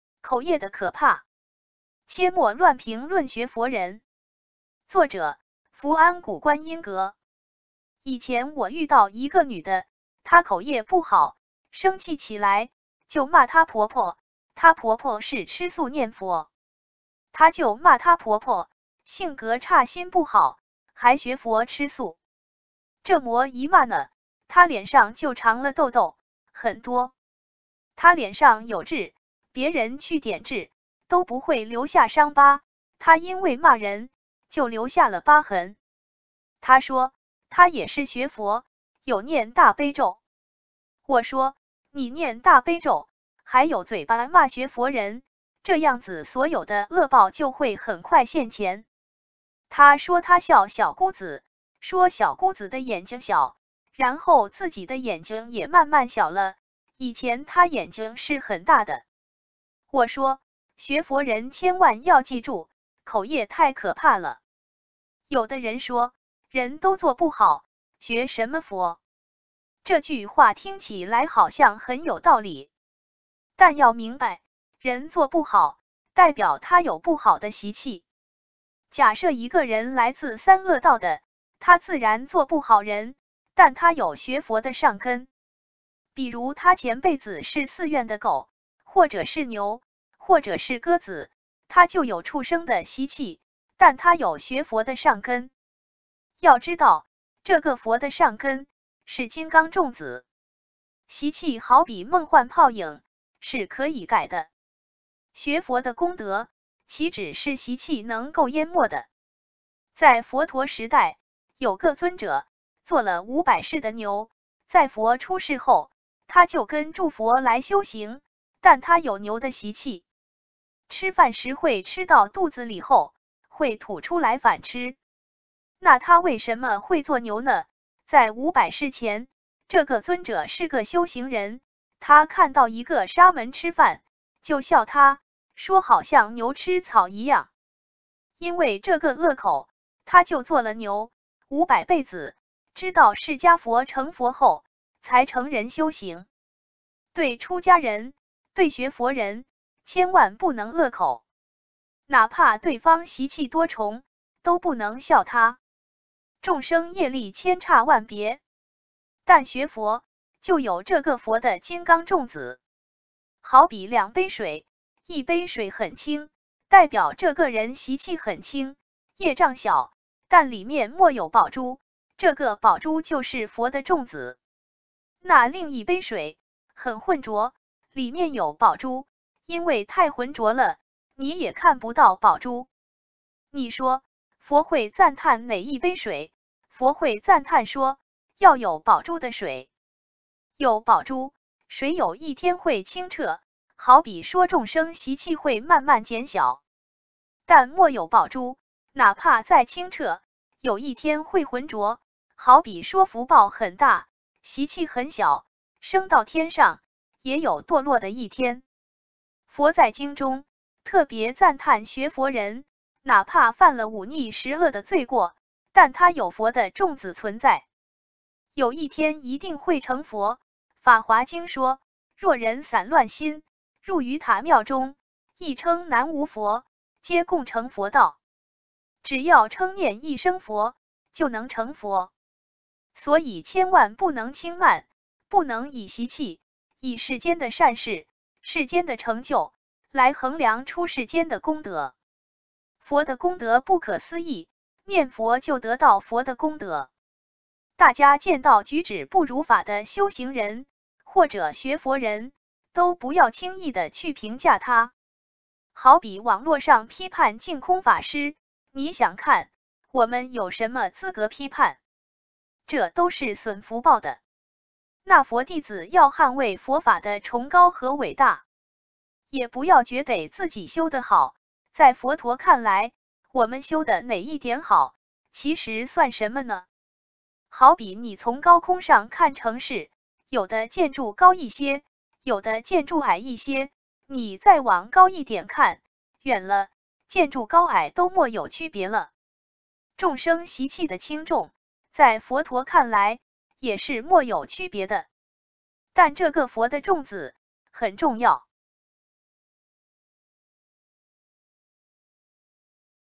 口業的可怕！切莫亂評論學佛人！
作者：福安古觀音閣

以前我遇到一個女的，她口業不好，生氣起來，就罵她婆婆，她婆婆是吃素念佛。她就罵她婆婆，性格差心不好，還學佛吃素？這麼一罵呢，她臉上就長了痘痘，很多。她臉上有痣，別人去點痣，都不會留下傷疤，她因為罵人，就留下了疤痕。

她說，她也是學佛，有念大悲咒。我說，你念大悲咒，還有嘴巴罵學佛人，這樣子所有的惡報就會很快現前。她說她笑小姑子，說小姑子的眼睛小，然後自己的眼睛也慢慢小了，以前她眼睛是很大的。我說，學佛人千萬要記住，口業太可怕了。

有的人說，人都做不好，學什麼佛？這句話聽起來好像很有道理。但要明白，人做不好，代表他有不好的習氣。假設一個人來自三惡道的，他自然做不好人，但他有學佛的善根。比如他前輩子是寺院的狗，或者是牛，或者是鴿子，他就有畜生的習氣，但他有學佛的善根。要知道，這個佛的善根，是金剛種子。習氣好比夢幻泡影，是可以改的。學佛的功德，豈止是習氣能夠淹沒的。

在佛陀時代，有個尊者，做了五百世的牛，在佛出世後，他就跟著佛來修行，但他有牛的習氣。吃飯時會吃到肚子裡後，會吐出來反吃。那他為什麼會做牛呢，在五百世前，這個尊者是個修行人，他看到一個沙門吃飯，就笑他，說好像牛吃草一樣。因為這個惡口，他就做了牛，五百輩子，知道釋迦佛成佛後，才成人修行。

對出家人，對學佛人，千萬不能惡口。哪怕對方習氣多重，都不能笑他。眾生業力千差萬別。但學佛，就有這個佛的金剛種子。好比兩杯水，一杯水很清，代表這個人習氣很清，業障小，但裡面沒有寶珠，這個寶珠就是佛的種子。

那另一杯水，很渾濁，裡面有寶珠，因為太渾濁了，你也看不到寶珠。你說，佛會讚歎哪一杯水，佛會讚歎說，要有寶珠的水。有寶珠，水有一天會清澈，好比說眾生習氣會慢慢減小。但沒有寶珠，哪怕再清澈，有一天會渾濁，好比說福報很大，習氣很小，升到天上，也有墮落的一天。

佛在經中，特別讚歎學佛人，哪怕犯了五逆十惡的罪過，但他有佛的種子存在。有一天一定會成佛，法華經說，若人散亂心，入於塔廟中，一稱南無佛，皆共成佛道。只要稱念一聲佛，就能成佛。所以千萬不能輕慢，不能以習氣，以世間的善事，世間的成就，來衡量出世間的功德。佛的功德不可思議，念佛就得到佛的功德。

大家見到舉止不如法的修行人，或者學佛人，都不要輕易的去評價他。好比網絡上批判淨空法師，你想看，我們有什麼資格批判。這都是損福報的。那佛弟子要捍衛佛法的崇高和偉大。也不要覺得自己修的好，在佛陀看來，我們修的哪一點好，其實算什麼呢？好比你從高空上看城市，有的建築高一些，有的建築矮一些，你再往高一點看，遠了，建築高矮都沒有區別了。眾生習氣的輕重，在佛陀看來，也是沒有區別的。但這個佛的種子，很重要。